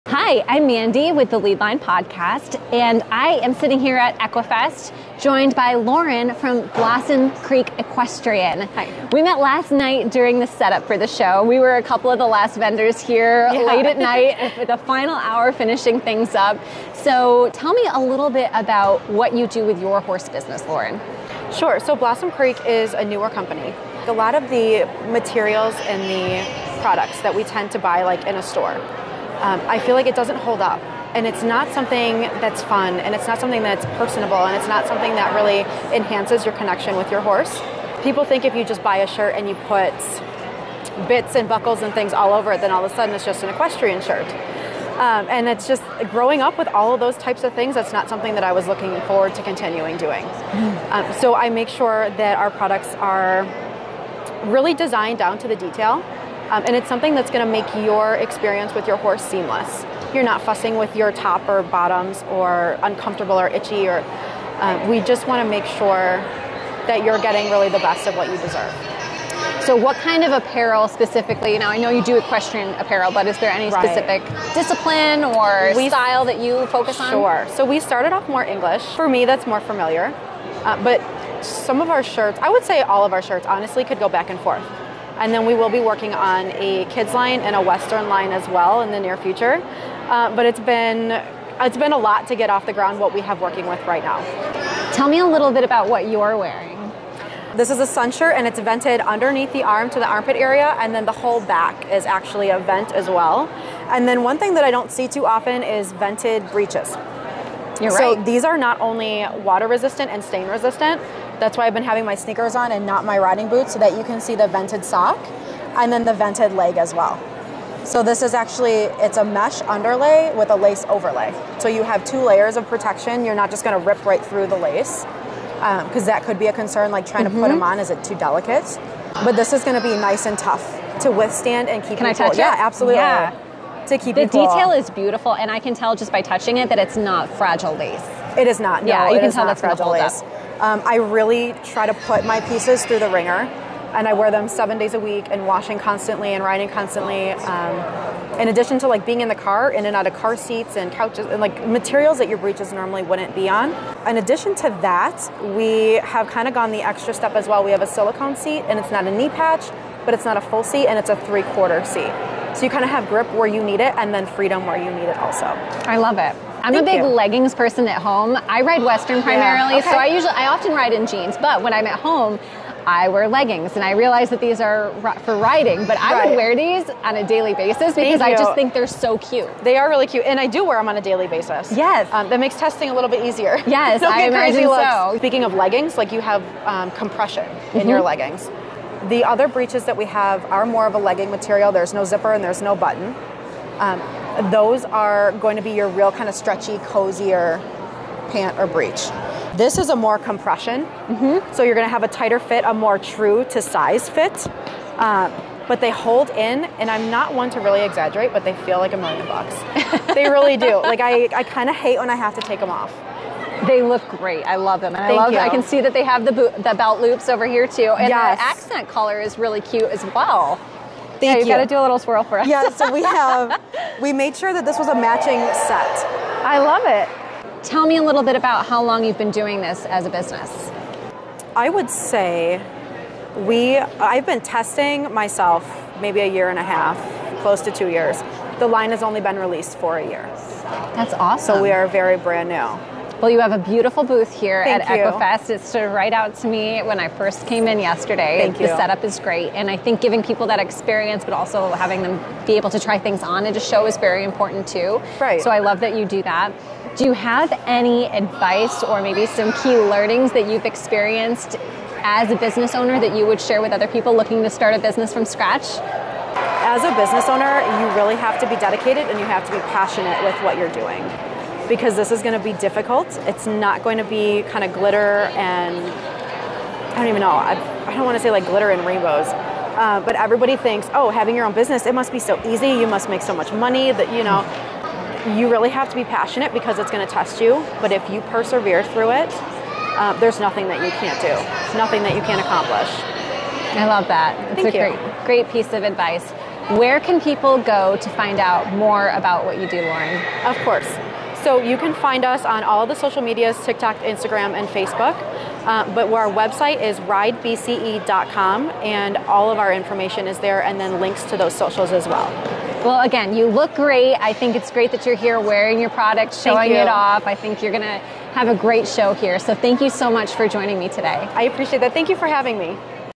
Interview with Blossom Creek Equestrian at WNY Equifest - The Leadline Podcast
Mandee Flanders of The Leadline Podcast interviews Lauren of Blossom Creek Equestrian at WNY Equifest in Hamburg, NY.